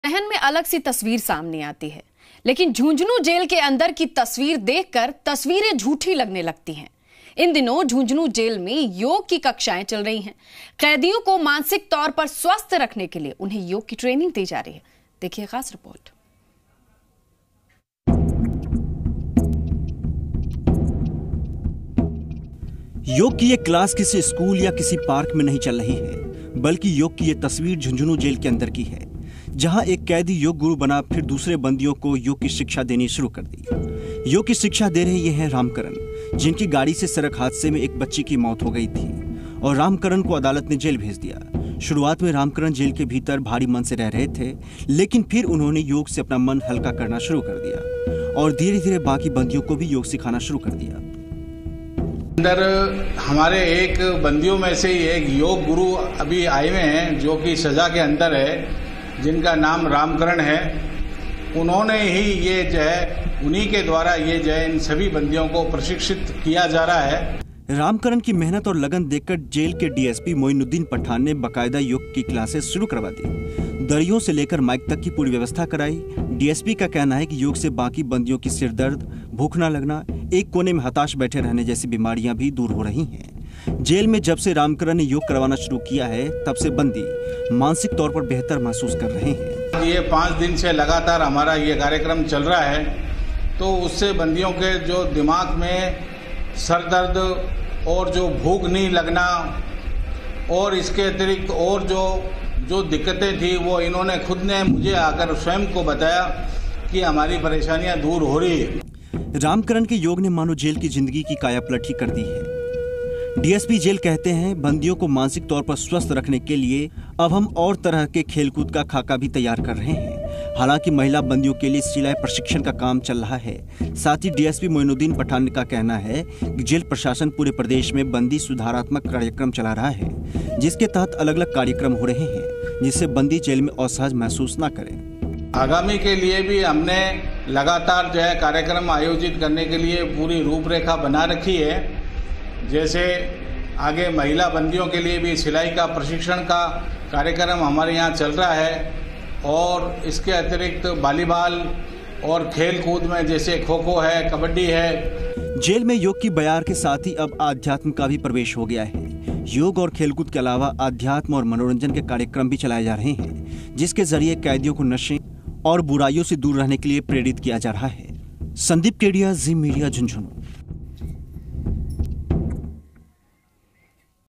झुंझुनू की जेल में योग
जेल का नाम सुनकर ही दिल में ना जाने कैसी-कैसी तस्वीर सामने आने लगती है...लेकिन झुंझुनू की जेल के अन्दर की तस्वीर देखकर तस्वीरें झूठी लगने गलती है...इन दिनों झुंझुनू जेल में योग की कक्षाएं चल रही है...कैदियों को मानसिक तौर से स्वस्थ्य रखने के लिए उन्हे योग सिखाया जा रहा है...